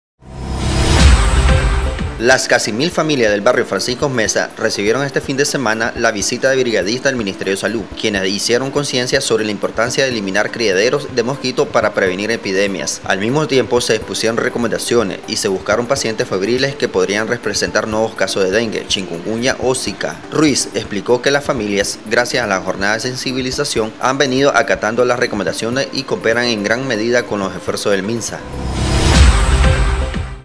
Promueven mayor conciencia para prevenir epidemias en el barrio Francisco Meza
Las casi mil  familias del barrio Francisco Meza de Meza recibieron este fin de semana, la visita de brigadistas del Ministerio de Salud, quienes hicieron conciencia sobre la importancia de eliminar criaderos de mosquitos para prevenir epidemias.

Al mismo tiempo, se expusieron recomendaciones y se buscaron pacientes febriles que podrían representar nuevos casos de dengue,  chickungunya o zica.